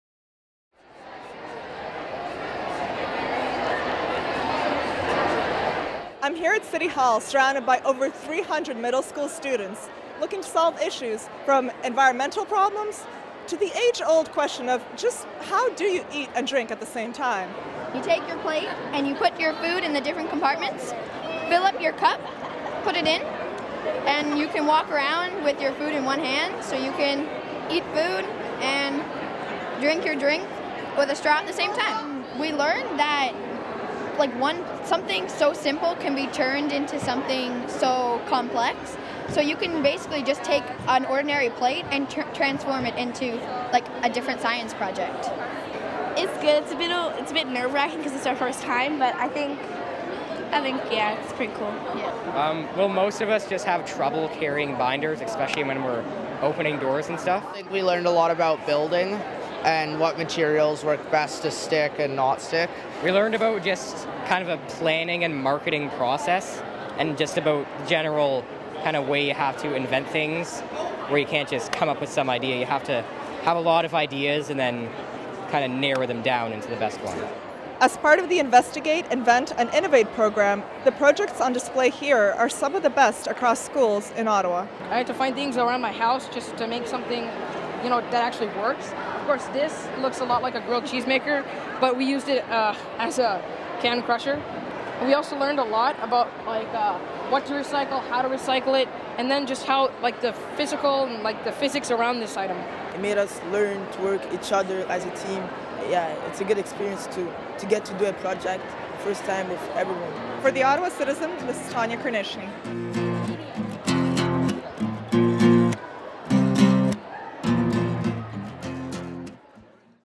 Ottawa middle school students show off their inventions
Investigate! Invent! Innovate! Invention Convention is a nationally-run, hands-on program integrating science, math and technology into curriculum for grades 7 and 8. Students have been working hard on inventing products or processes to solve a variety of common, daily problems based on concepts they learn in class.Video by Darren Brown/The Ottawa Citizen